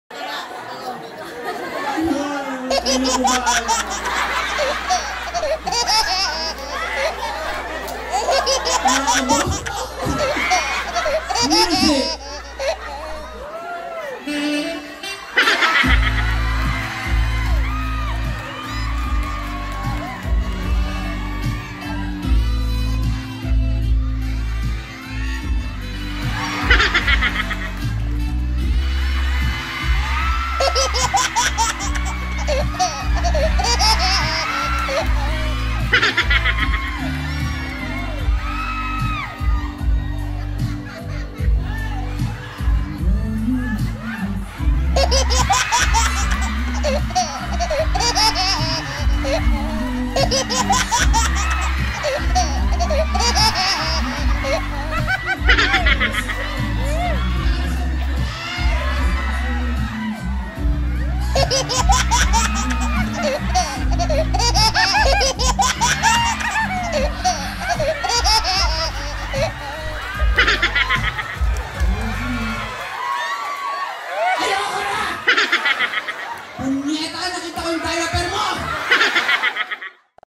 TAWANAN PA MORE!!!***WACKY KIRAY SINUNGGABAN NG BABAE SA CEBU***SA SHOW N'YA AT CASINO FILIPINO CEBU
Nakakatuwang panoorin si Wacky Kiray, subrang tawa namin sa kanyang galing ng pagpapatawa at Casino Filipino Cebu Concert on January 04,2020.